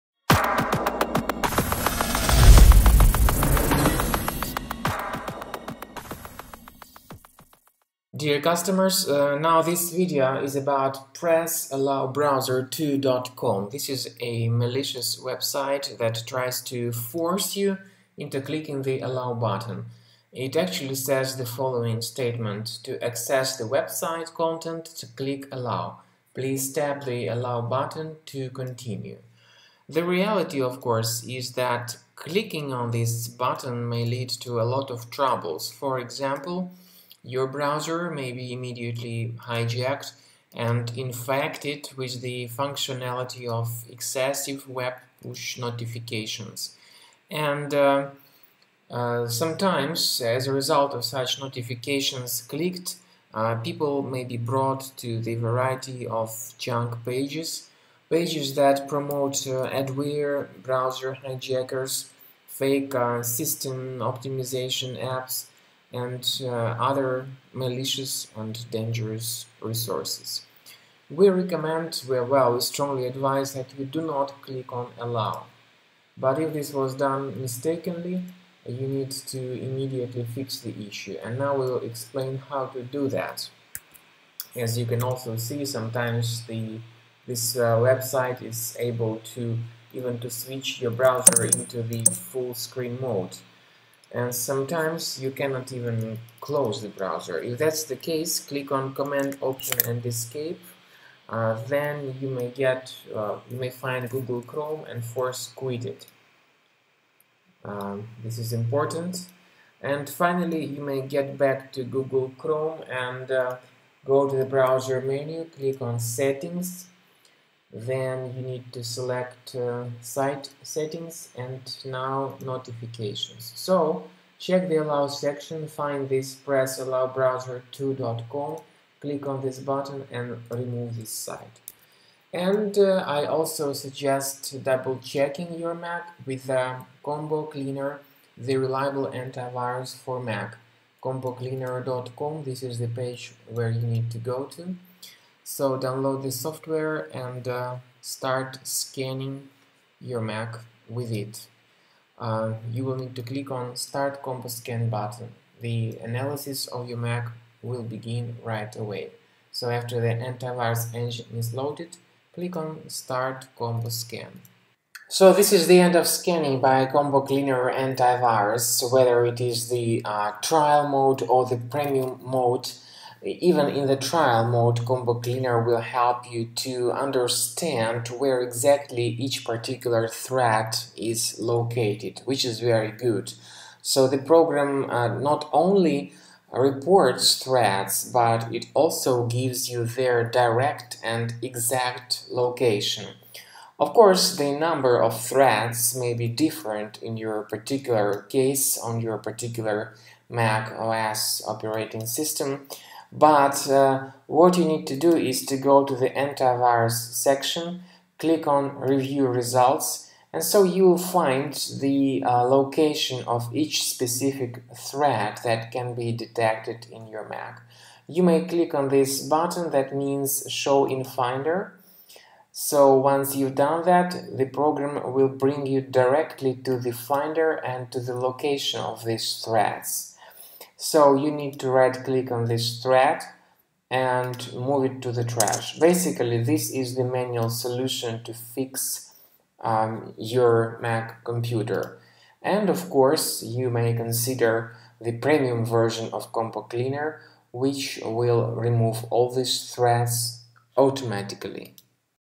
Press-allow-browser2.com push notifications (Removal Guide).
Press-allow-browser2.com is a junk site that shows repeated web push notifications. Remove them all with Combo Cleaner Antivirus or manually based on the recommendations in the video.